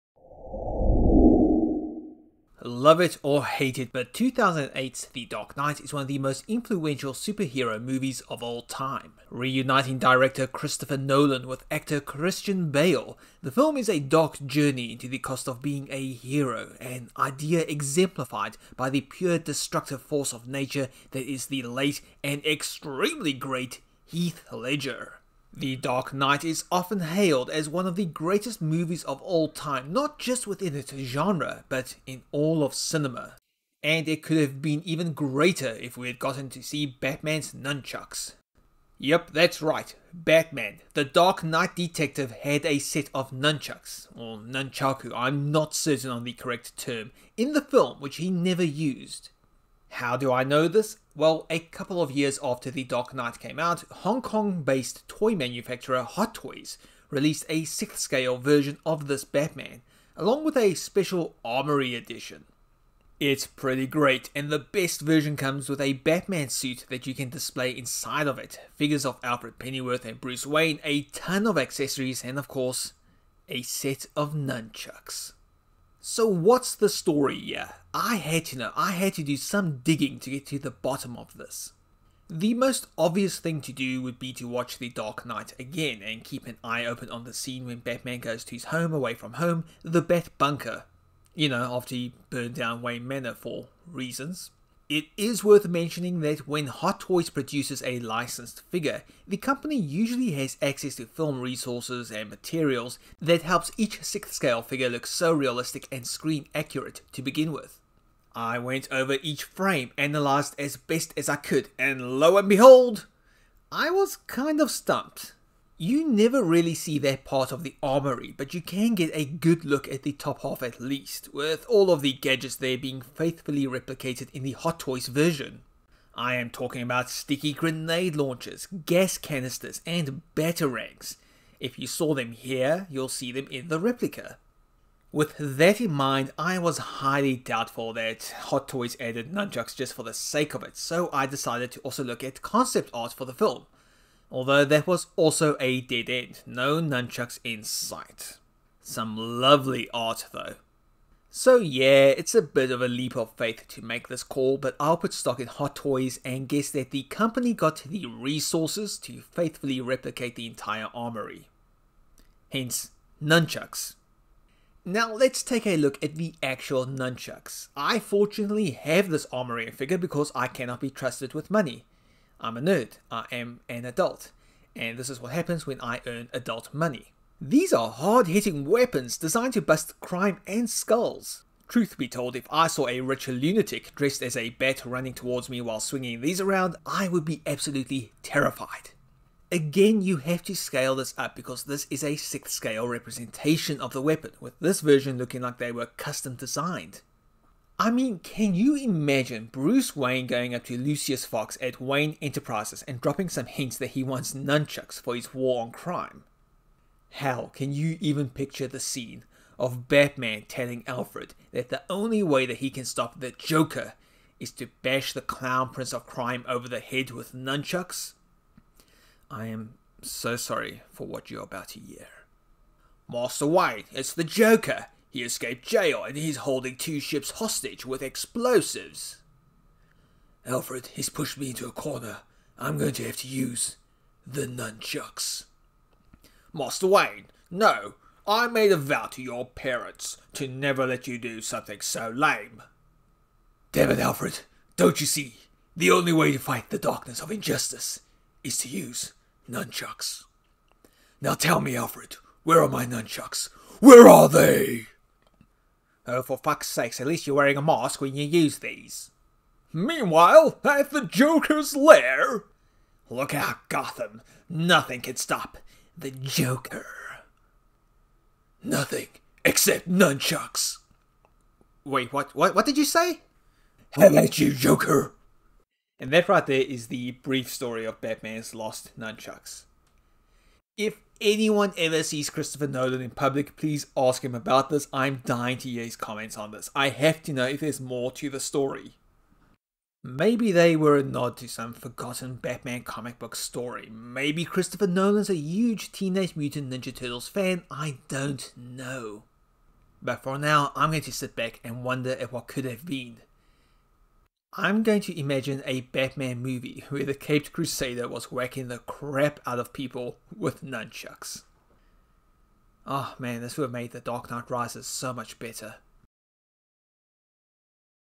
What Happened To Batman's Nunchucks?
This is the true story--and kind of cringe--tale of Batman's missing nunchucks.